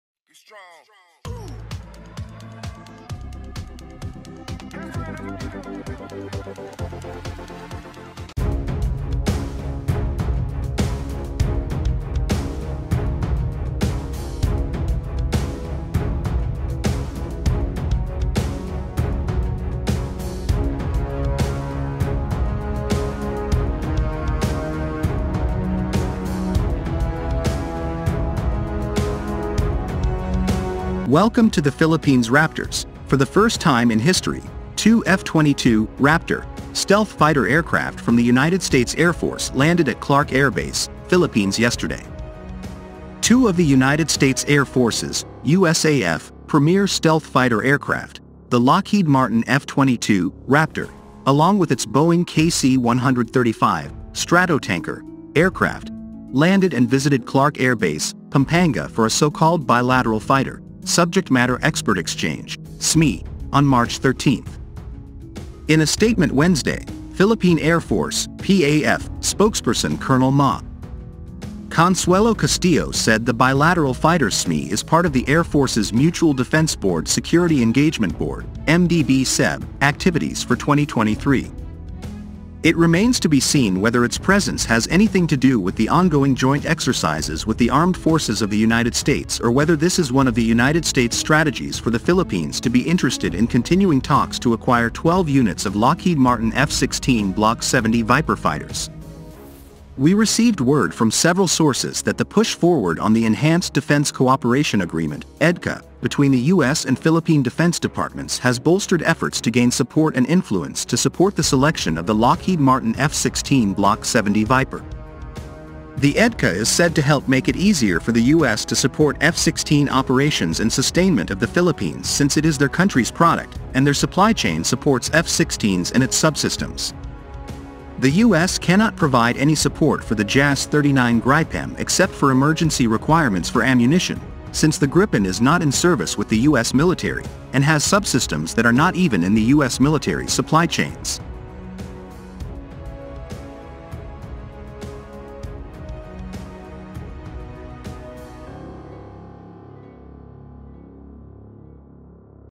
Two F-22 "Raptor" Stealth Fighters Come to Philippines Promotional Steps of US F-16 Block 70 Viper
Welcome to the Philippines Raptors, For the first time in history, two F-22 "Raptor" Stealth Fighter Aircraft from the United States Airforce landed at Clark Air Base, Philippines yesterday.

Two of the United States Air Force's (USAF) premier stealth fighter aircraft, the Lockheed Martin F-22 "Raptor" along with its Boeing KC-135 "Stratotanker" aircraft, landed and visited Clark Air Base, Pampanga for a so-called bilateral fighter "subject matter expert exchange" (SMEE) on March 13.

In a statement Wednesday, Philippine Air Force (PAF) spokesperson Col. Ma. Consuelo Castillo said the bilateral fighter SMEE is part of the Air Force's Mutual Defense Board-Security Engagement Board (MDB-SEB) activities for 2023.